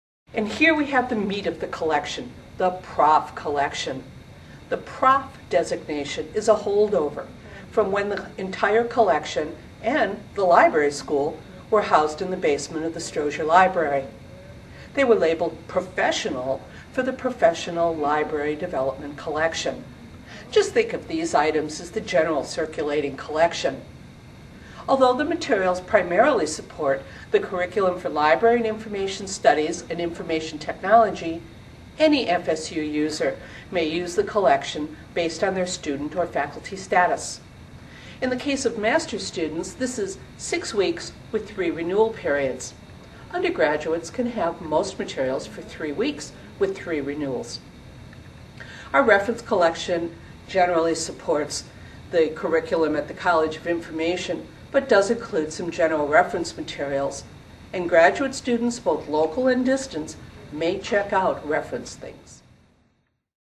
Goldstein Library's Professional Collection
This video highlights the Goldstein's collection of Library & Information Science and Information Technology Professional Literature.  Narrated by Goldstein Director, Pam Doffek